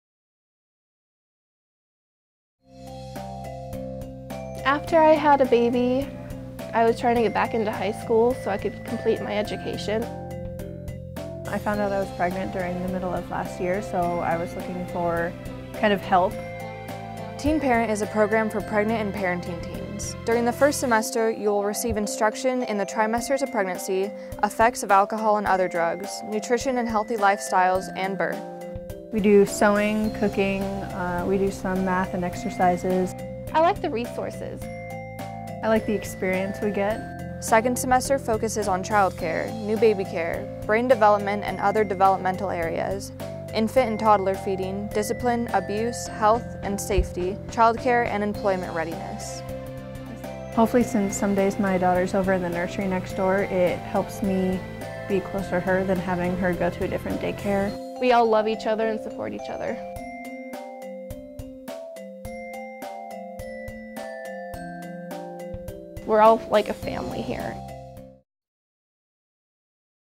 Teen Parenting 2016-2017
This course is for high school age students who are or are about to become parents. Enrollment is open throughout the year and day care is provided on a space-available basis.